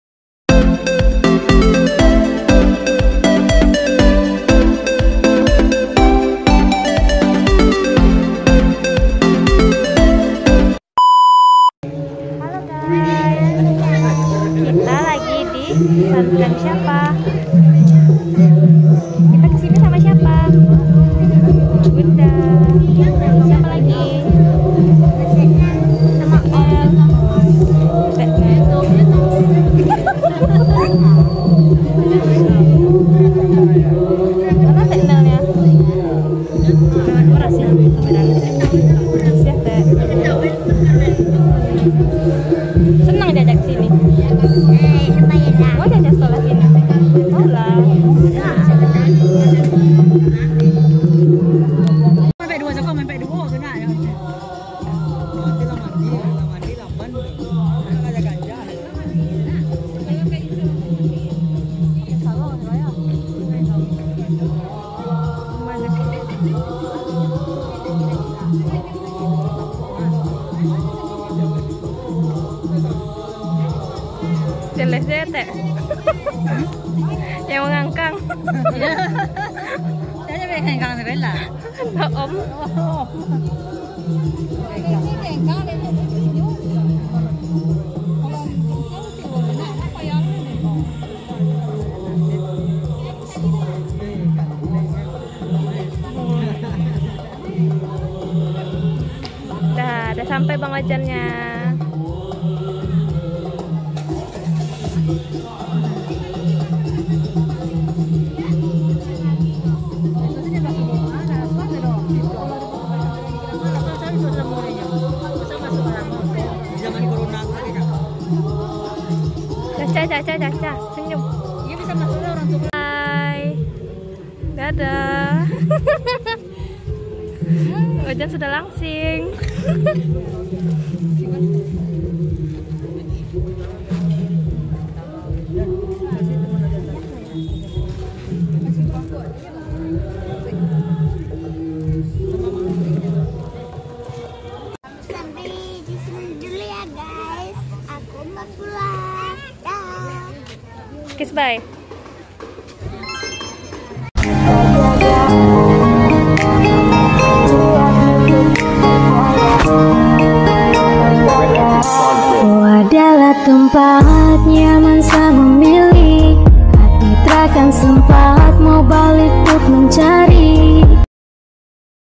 Vlog#6 Suasana Sore di Pondok Pesantren
ini video pertama jenguk ojan dipesantren (15 Agustus 2021)
ada kak dila, mama ojan, ayah ojan, makwin & caca
selamat menonton